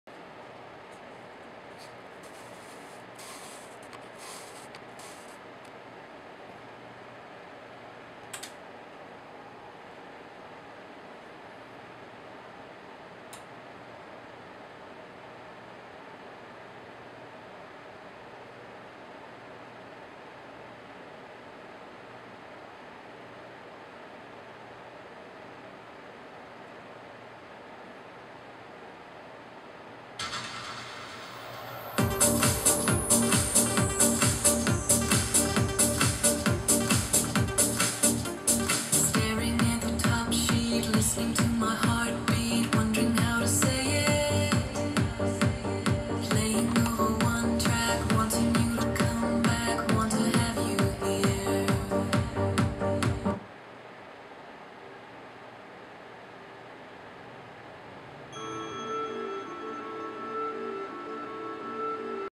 Rooted Nook as a remote for Onkyo
testing nook as a remote for a TX-NR609... no opinion on music choice.